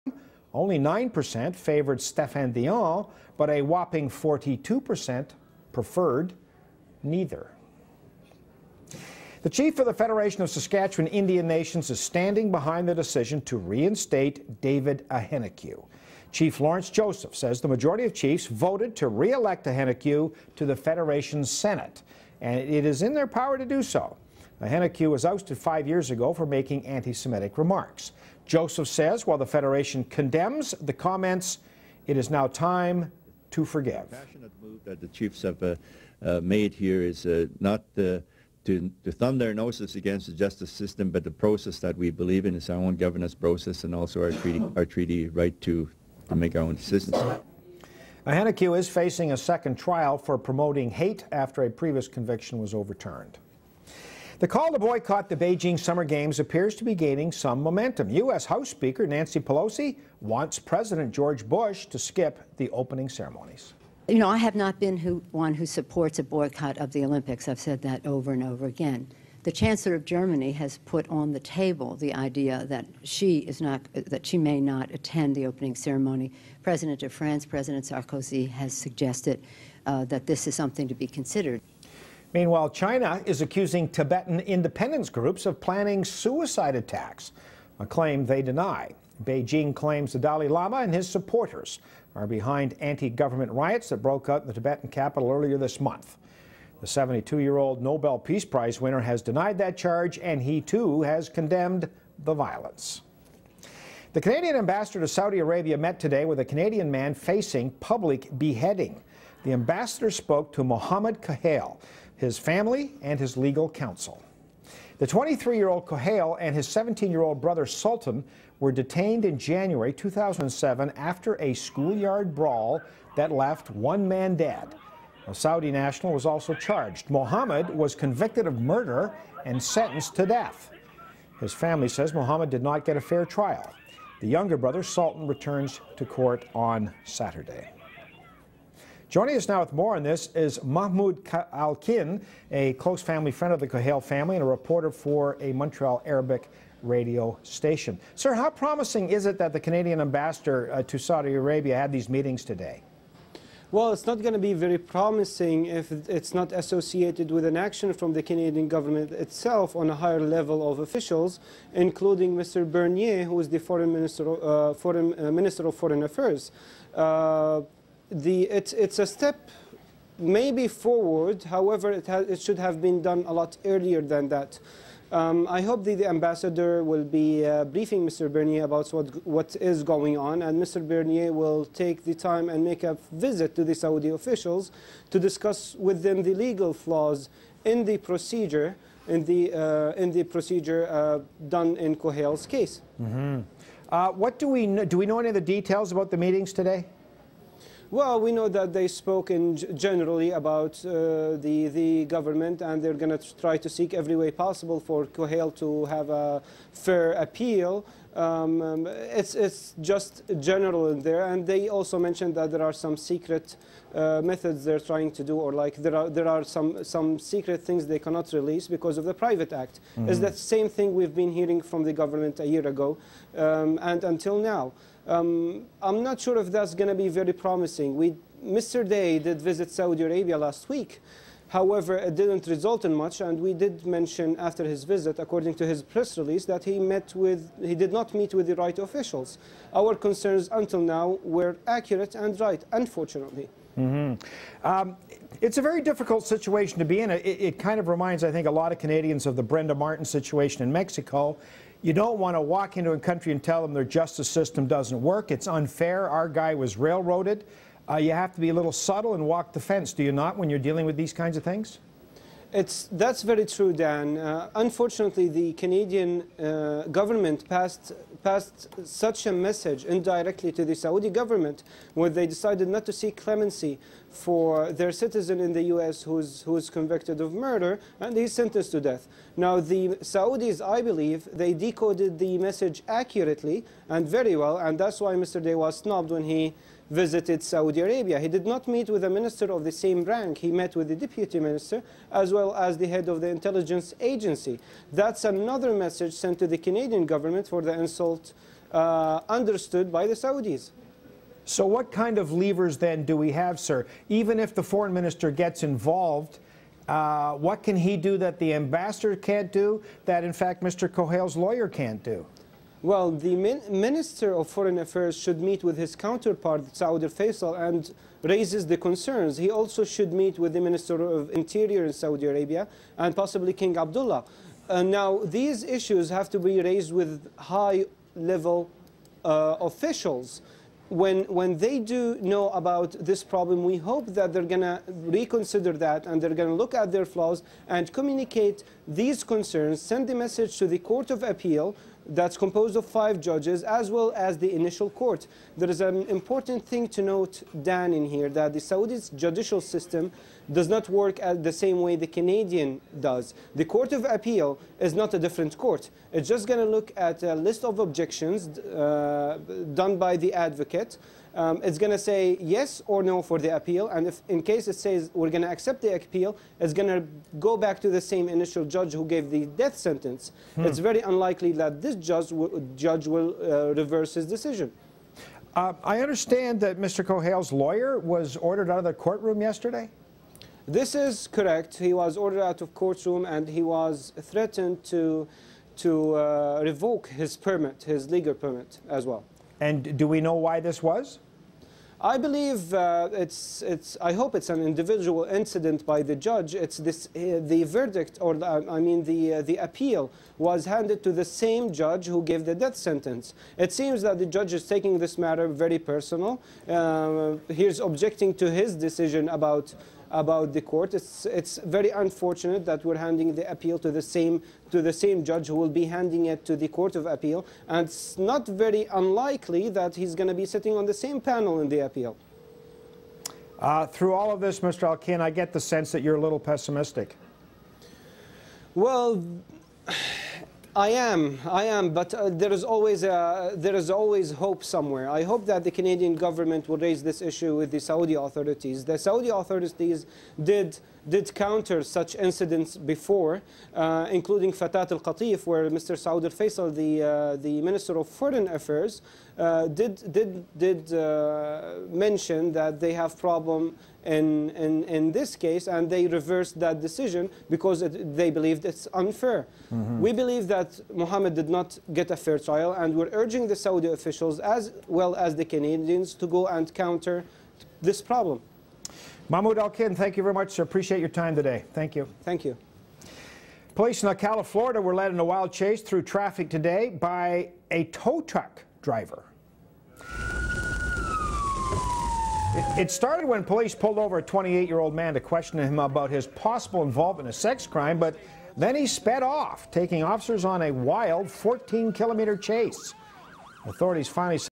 CTV Inervew about Muhammad Kohail April 1st
CTV live interview on April 1st after the Canadian Ambassador Visited Muhammad Kohail in his Jail